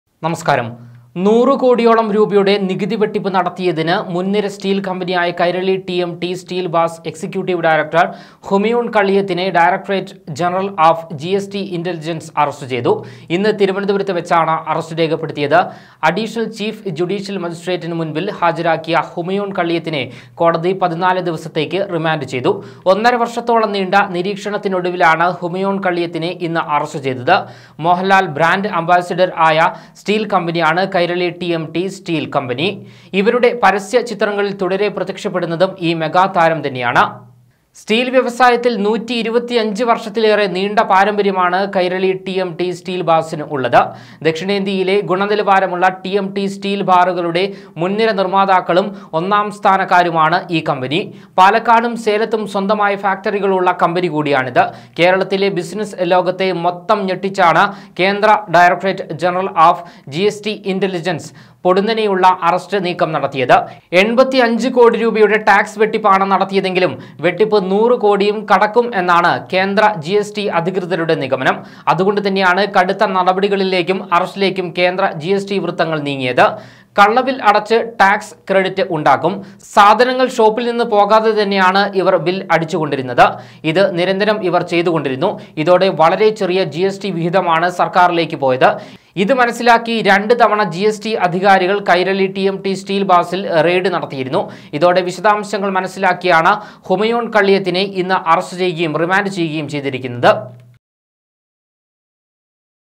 കൈരളി ടി.എം ടി സ്റ്റീൽ ബാർസ് എക്‌സിക്യൂട്ടിവ് ഡയറക്ടർ അറസ്റ്റിൽ I KAIRALI TMT
കള്ളബിൽ അടിച്ച് നൂറുകോടിയോളം വരുന്ന നികുതി തട്ടിപ്പ്; കുരുങ്ങിയത് കൈരളി ടി.എം ടി സ്റ്റീൽ ബാർസ് എക്‌സിക്യൂട്ടിവ് ഡയറക്ടർ ഹുമയൂൺ കള്ളിയത്ത്; മോഹൻലാൽ ബ്രാൻഡ് അംബാസഡർ ആയ കമ്പനിയുടെ എക്‌സിക്യൂട്ടിവ് ഡയറക്ടറെ അറസ്റ്റ് ചെയ്തത് ഡയറക്ടറെറ്റ് ജനറൽ ഓഫ് ജിഎസ്ടി ഇന്റലിജൻസ്; കേരളത്തിലെ ബിസിനസ് വൃത്തങ്ങളെ ഞെട്ടിച്ച അറസ്റ്റ് ഇങ്ങനെ